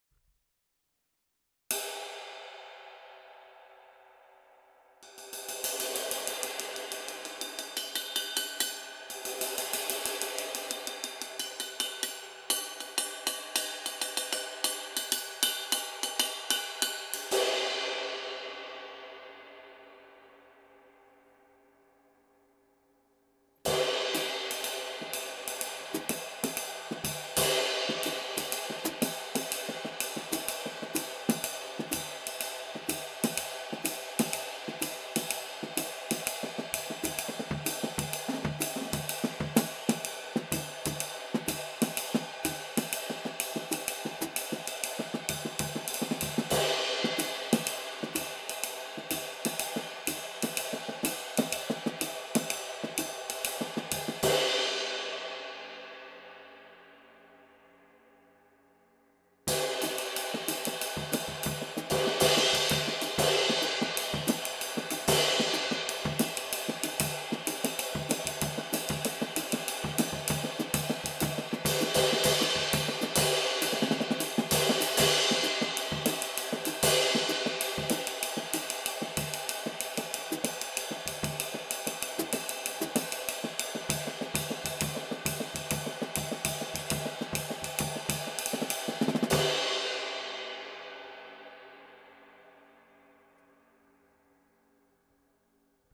22 Rustico POF Ride, 2269g - Handcrafted cymbals by Craig Lauritsen
This cymbal was hammered and lathed, then over-hammered, patinated and then flat bit lathed and brush finished (top only). The profile is a little higher than most of the recent Rustico's. Most of the patina was removed with the flat faced bit, the remaining patina staying mainly in the concave over-hammering marks.

The pressed bell was also flattened with extensive hammering near the bridge.
The 'POF' is as much for my recollection as anything and denotes the patina, then over-hammering, then flat faced bit lathing method used to pre-age the bronze.